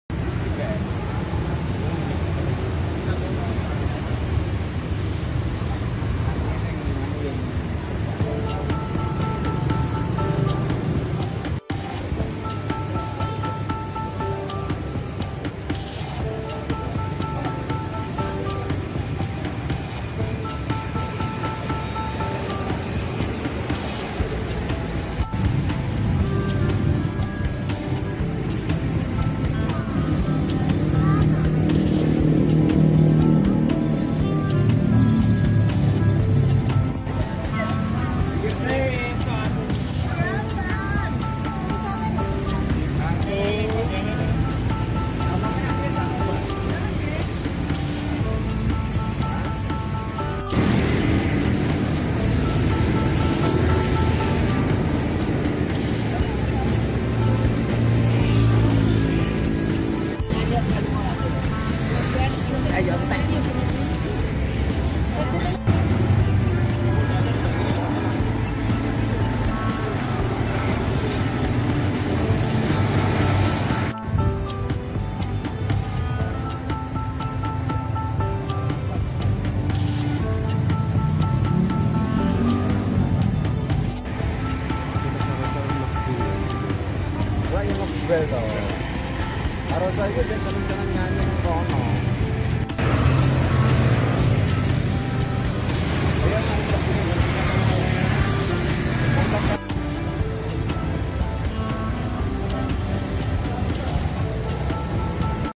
Suasana Taman Sengkaling UKM Malang Malam Hari || Primadona Refresing
Malam minggu  malam yang di tunggu tunggu untuk refresing sejenak di luar rumah  setelah selam 6 hari kerja kerja  mencari nafkah,  maka tidak ada salahnya Taman rekreasi Sengkaling jadi primadona untuk  tempat jalan jalan dan menghibur diri